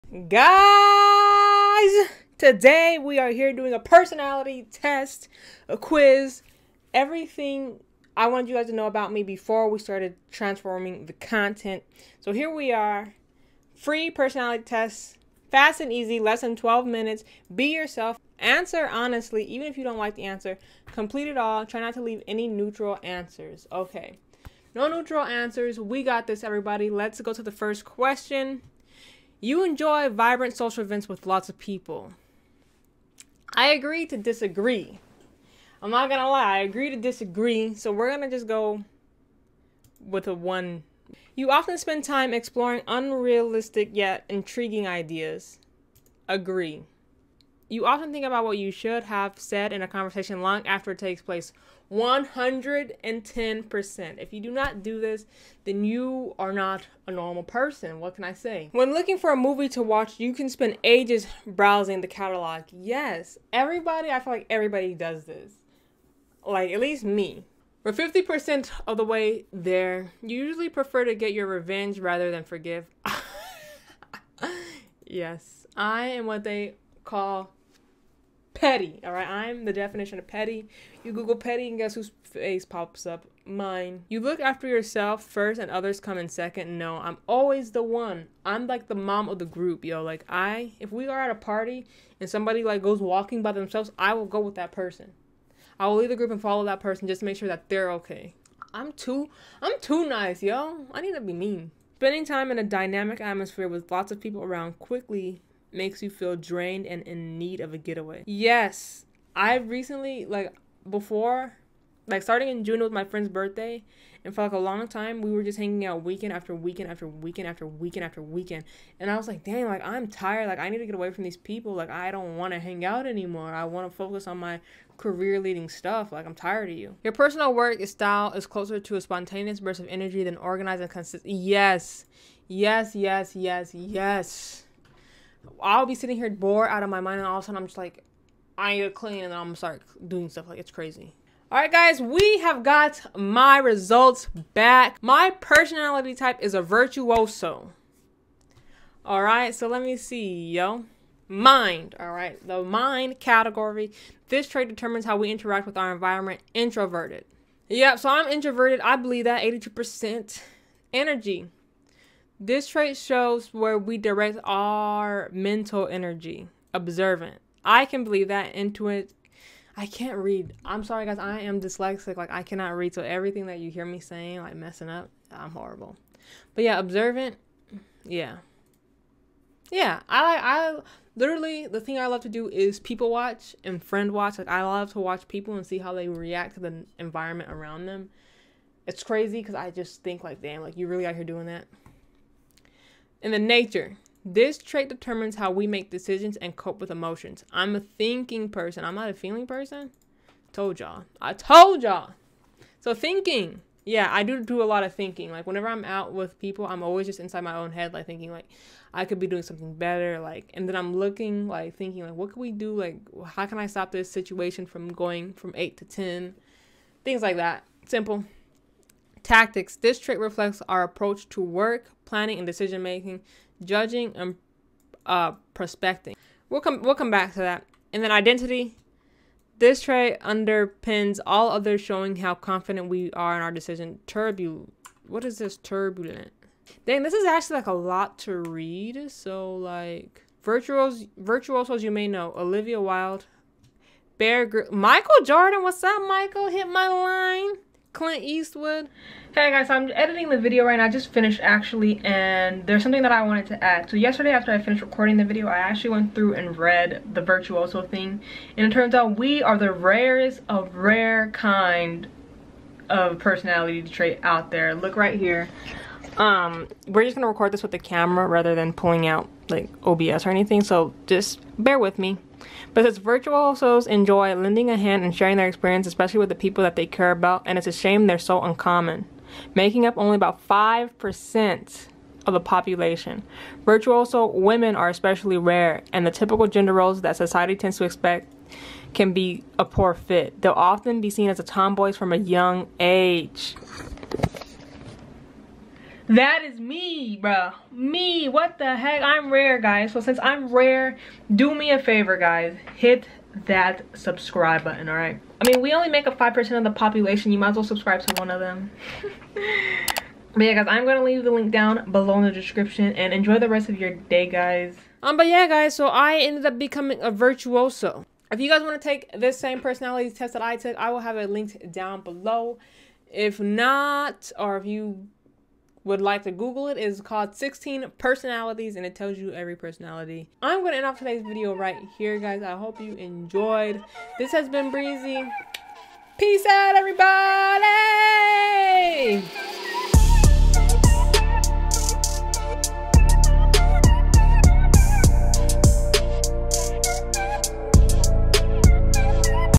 I am the RAREST TYPE of person in the WORLD
Enjoyed this video? Watch the latest video here - Isiah's Quest To Restore His Family | The Sims 4
7 Views - Mar 16, 2024

Guys I cannot believe I am the rarest type of personality in the world!